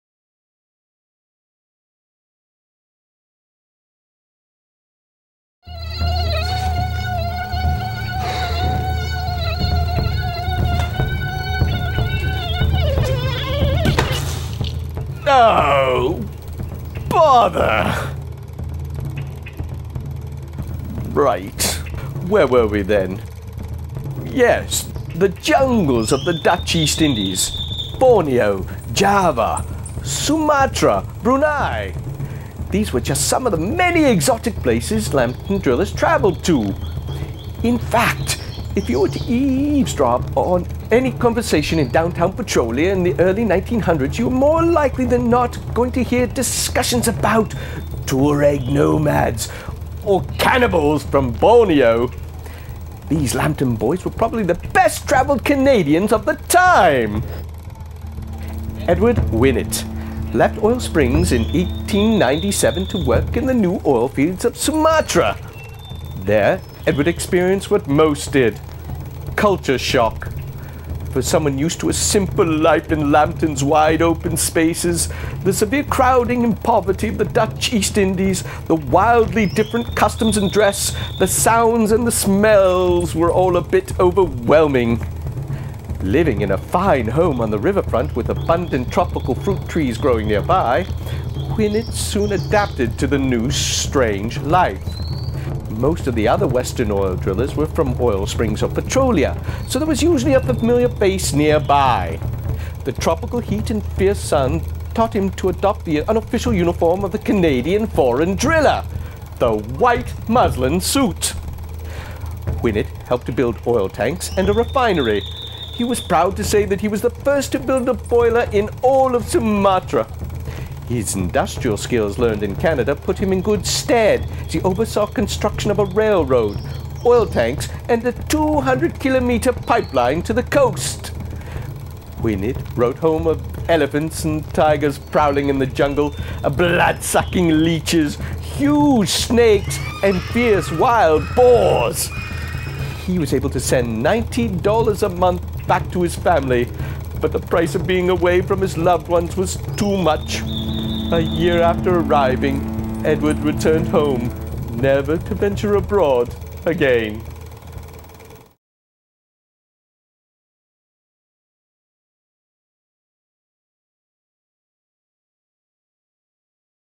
Edward Winnett à Bornéo
Extrait de l'exposition "International Driller Tent".
Produit par Avolution Multimedia pour le Oil Museum of Canada.